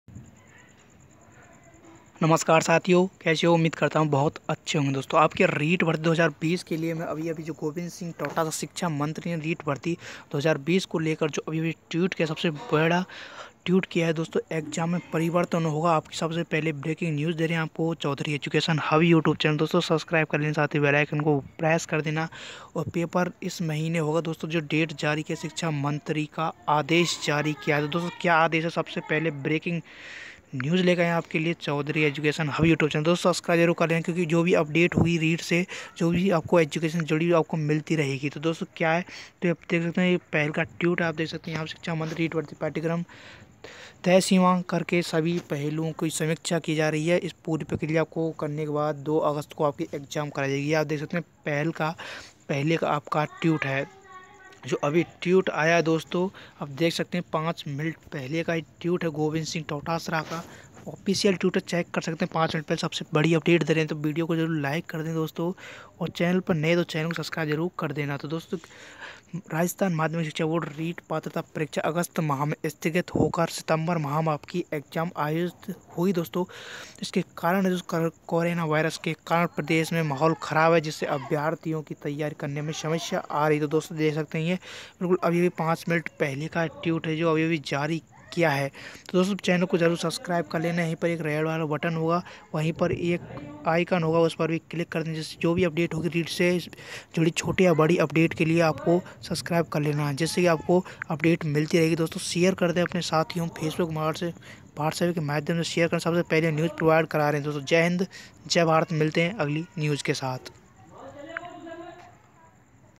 REET भर्ती 2020 ।। एग्जाम परिवर्तन ब्रेकिंग न्यूज़ पेपर इस महीने होगा ।। शिक्षा मंत्री का आदेश ।।
about this video : reet Bharti syllabus ki jankari Di hai.

Reet2020 ,
reetsyllabus+विज्ञप्ति,
reettodaybrakingnews
reet2020examdate examdatereet2020